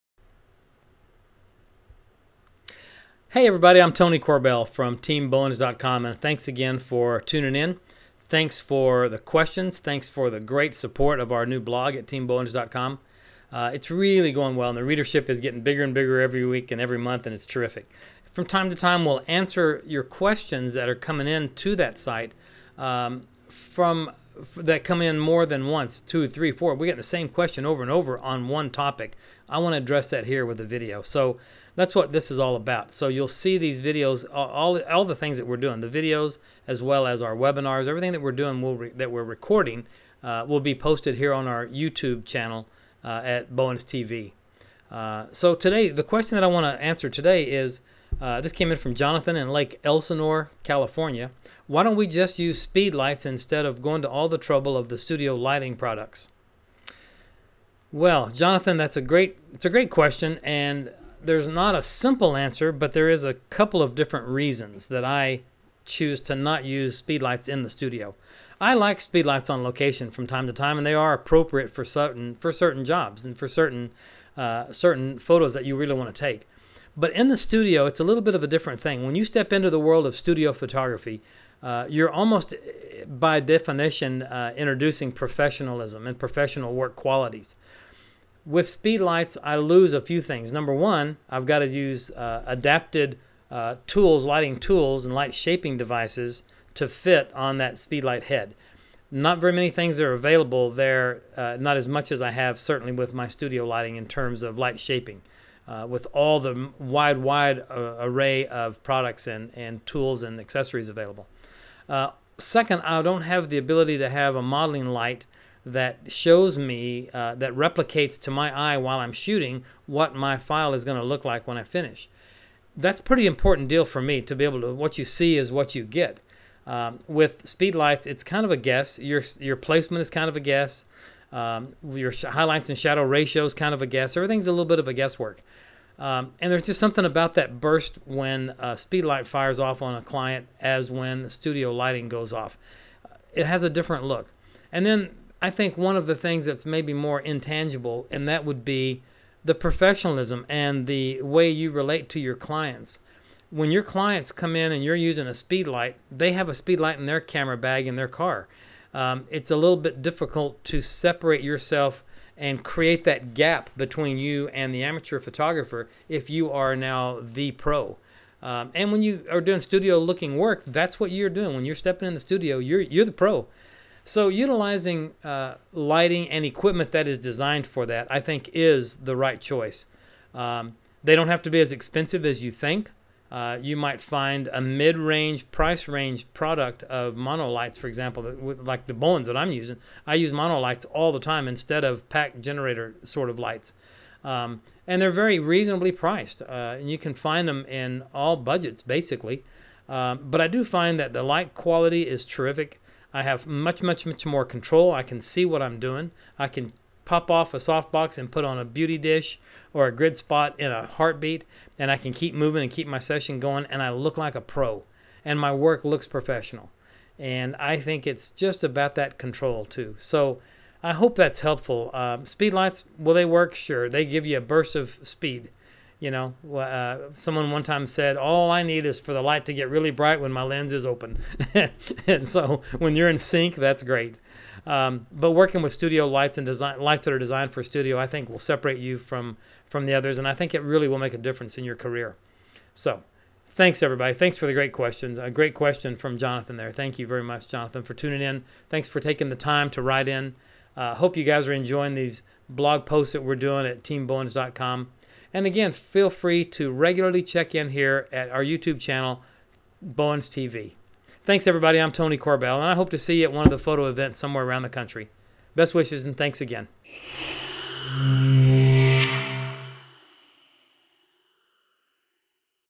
Ask TeamBowens: Why not Speedlights?
Tony Corbell compares Speedlights/Speedlites to studio flashes.

To ask one of our TeamBowens lighting experts your own question, go